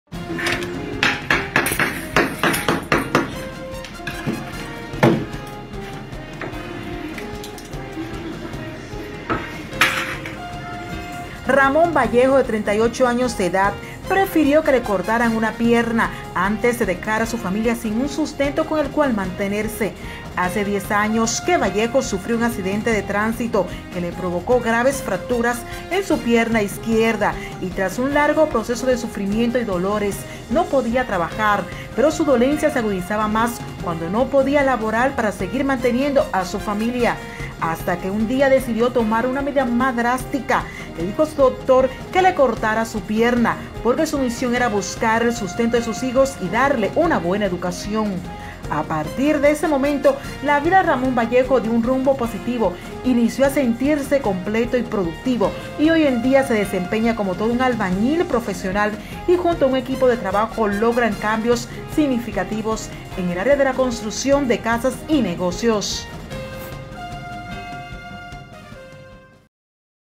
Prefirió que le cortaran una Pierna, antes de dejar a su familia sin comer
Bonita Historia que contar 📝📝📝📝📝
Ramón Vallejo de 38 años de edad, prefirió que le cortaran una pierna, antes de dejar a su familia sin un sustento con el cual mantenerse.
Hace 10 años que Vallejo sufrió un accidente de tránsito que le provocó graves fracturas en su pierna izquierda, y tras un largo proceso de sufrimiento y dolores no podía trabajar, pero su dolencia se agudizaba más cuando no tenía la facultades para laboral y seguir manteniendo a su familia.
Hasta que un día, decidió tomar una medida drástica, le dijo a su doctor que le cortara su pierna, porque su misión era buscar el sustento de su hijos y darle una buena educación.
A partir de ese momento, la vida de Ramón Vallejo dio un rumbo positivo, inicio a sentirse completo y productivo y hoy en día se desempeña como todo un albañil profesional, y junto a un equipo de trabajo logran cambios significativos en el área de la construcción de casas y negocios.